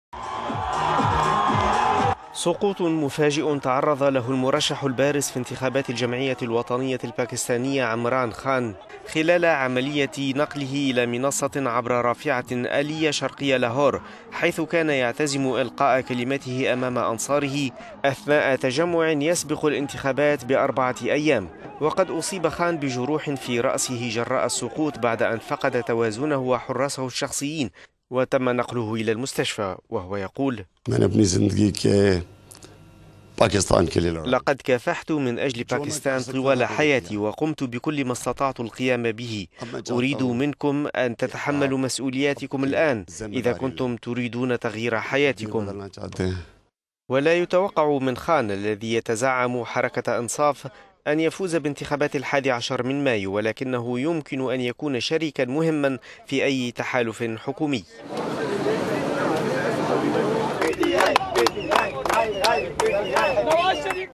إصابة الباكستاني عمران خان بجروح
سقوط مفاجئ تعرض له المرشح البارز في انتخابات الجمعية الوطنية الباكستانية عمران خان، خلال عملية نقله إلى منصة عبر رافعة آلية شرقي لاهور، حيث كان يعتزم القاء كلمته أمام أنصاره أثناء تجمع، يسبق الانتخابات بأربعة أيام.

وقد أصيب خان بجروح في رأسه، جراء السقوط بعد أن فقد توازنه وحراسه الشخصيين، وتم نقله الى المستشفى وهو ليس في حالة خطيرة بحسب الأطباء.

ويقول عمران خان الذي يتزعم حركة "إنصاف":
"لقد كافحت من أجل باكستان طوال حياتي، وقمت بكل ما استطعت القيام به. الآن أريد منكم أن تتحملوا مسؤولياتكم. إذا كنتم تريدون تغيير حياتكم فلتتحملوا مسؤولياتكم".

ولا يتوقع من خان أن يفوز بانتخابات الحادي عشر من مايو، ولكنه يمكن أن يكون شريكا مهما في أي تحالف حكومي.

وقد وعد بمكافحة التهرب الضريبي والفساد الذي استنزف البلاد.